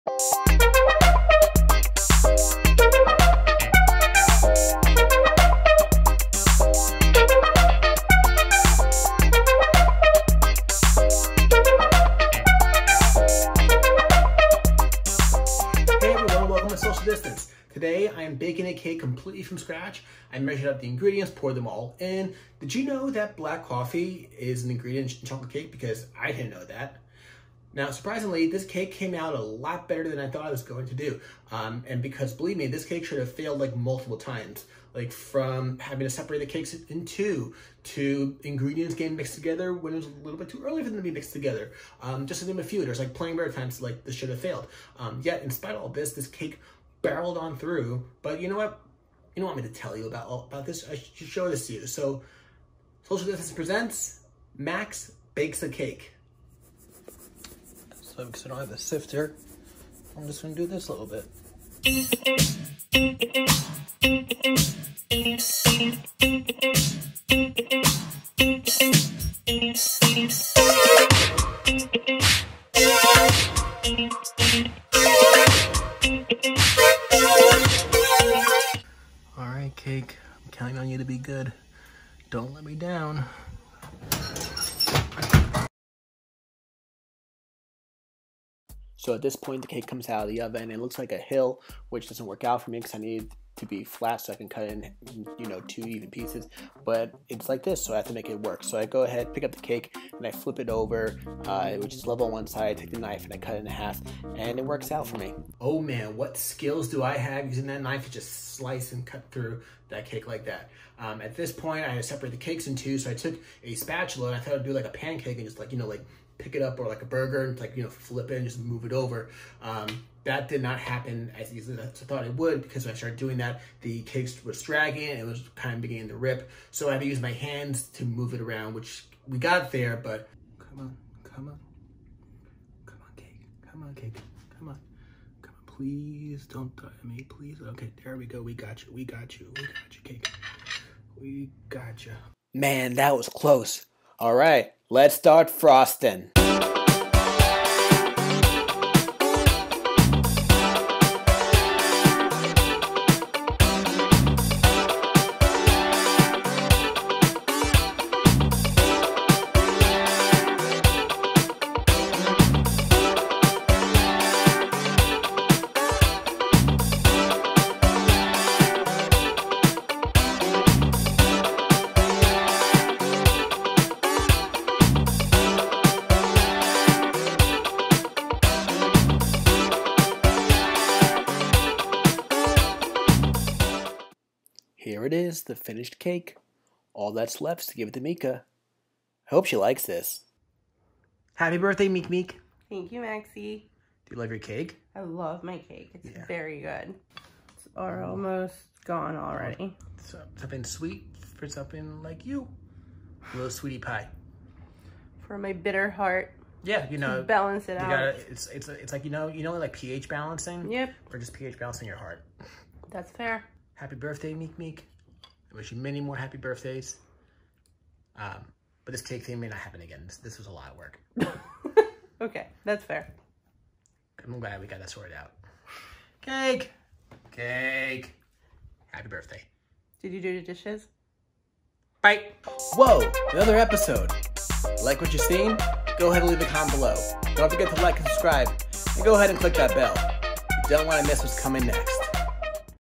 Social Distance Ep 35 - Max Bakes a Cake
It's Mikka's Birthday and I'm making her a cake from scratch.  Only thing is I've never done this before.  This will end well right?  Watch and find out.